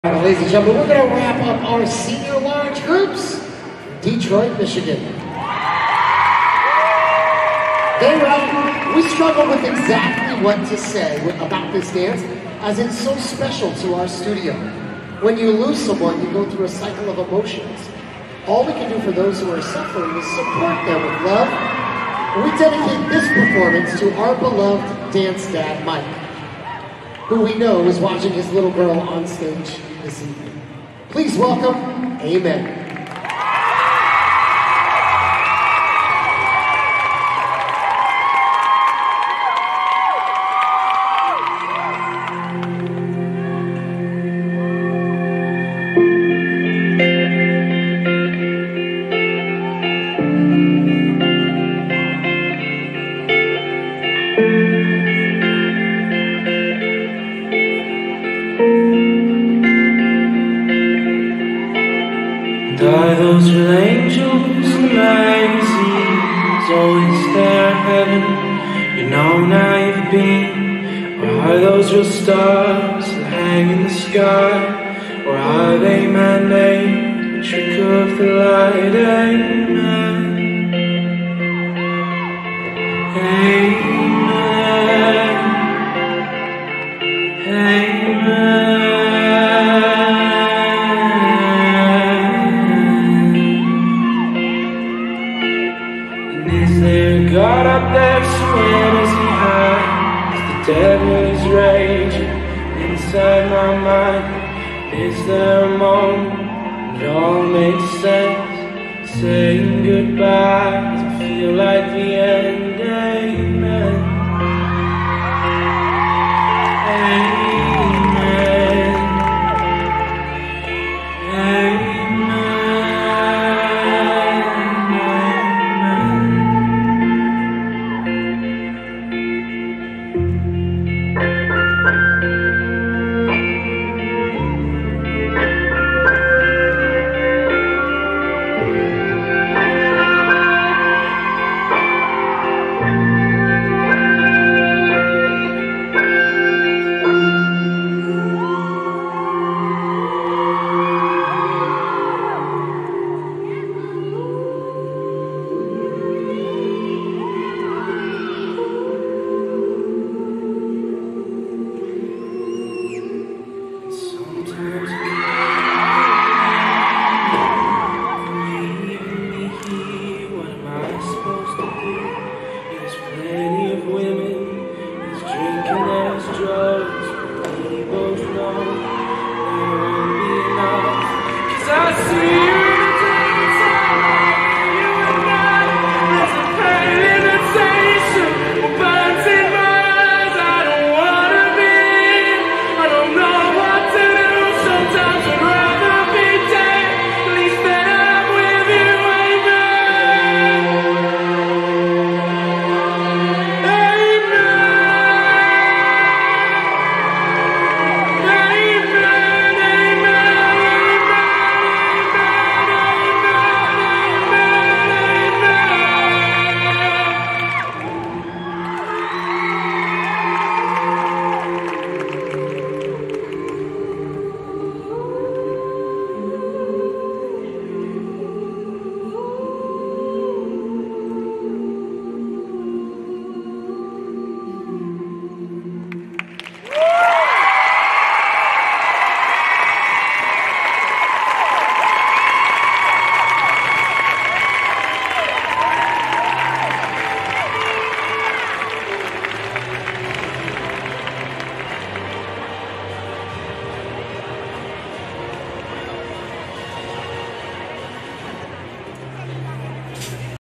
Big City Dance Center-AMEN-Sr, Large Group- National Victory Cup Winner
This amazing piece was undefeated all season
Choreographed by Mitchell Federan